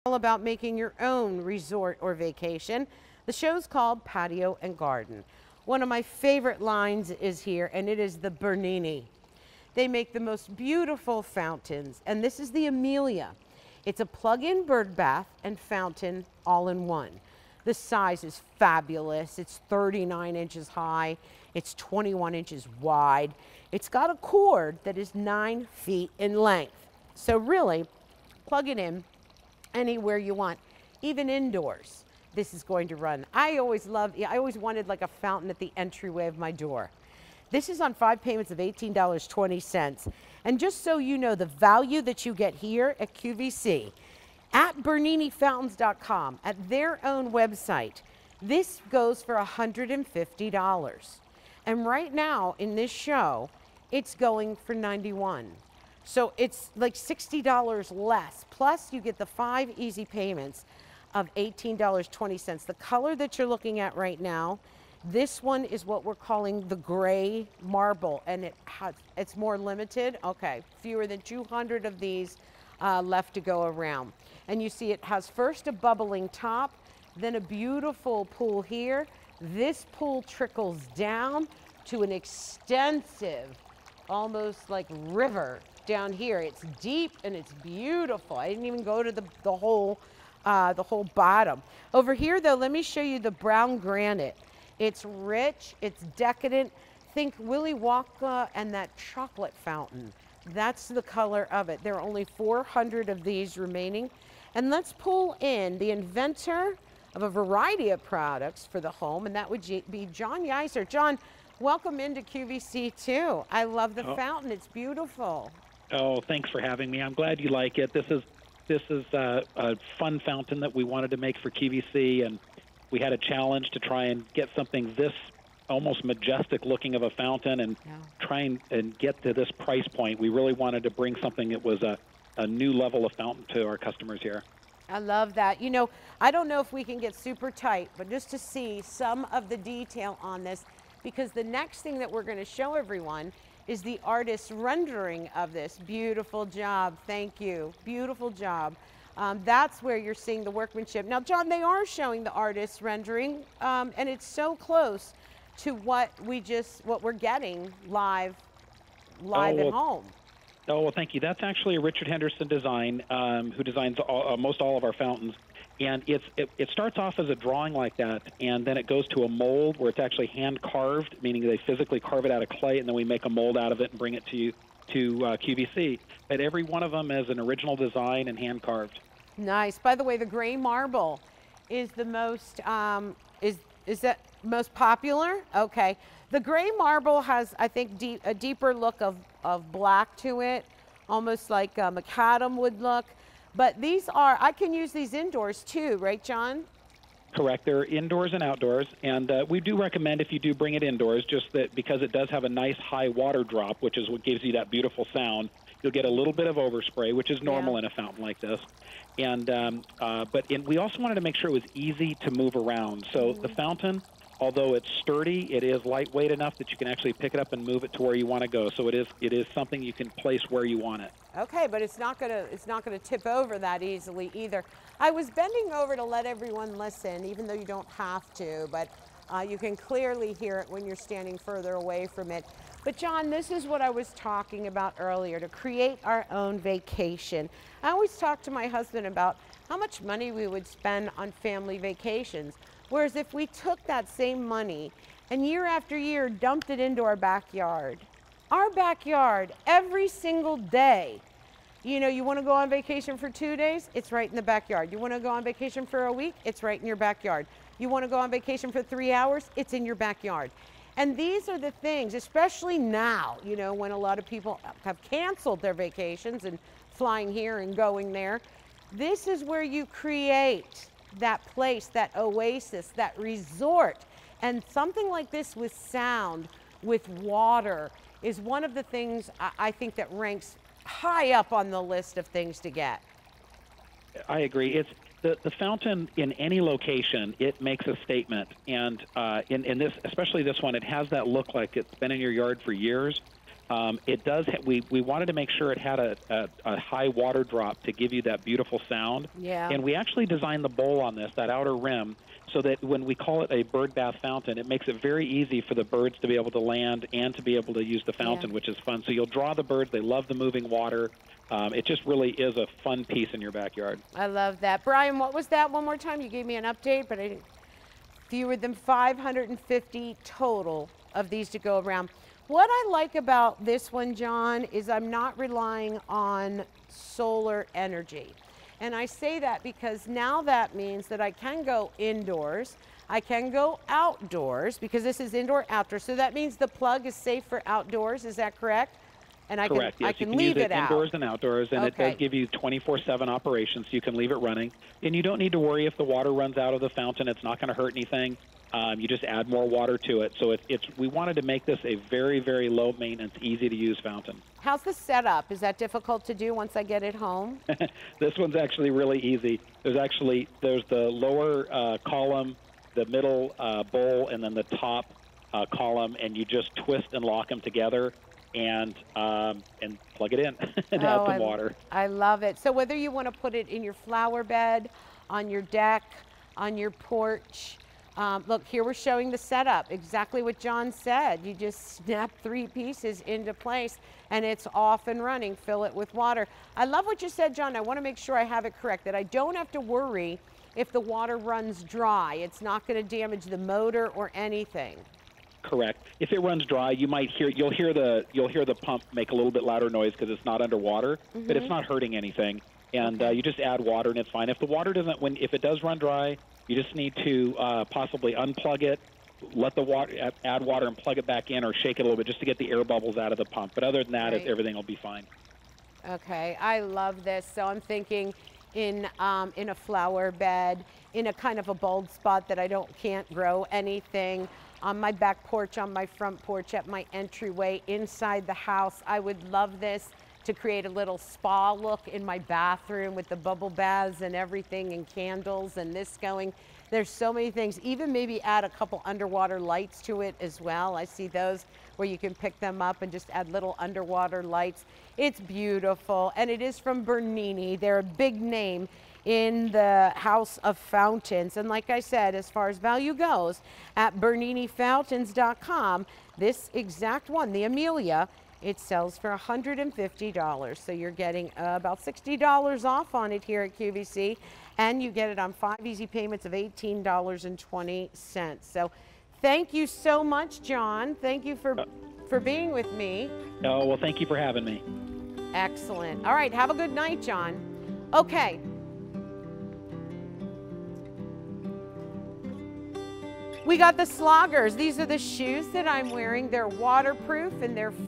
Bernini "The Emilia" Plug-In Birdbath and Fountain on QVC
Bernini "The Emilia" Plug-In Birdbath and Fountain

Bring a touch of Italian elegance to your backyard with this artist-designed birdbath. With its shallow bowl, gentle sloping edges, and circulating water, the fountain creates a relaxing space for you and all your feathered friends. From Bernini.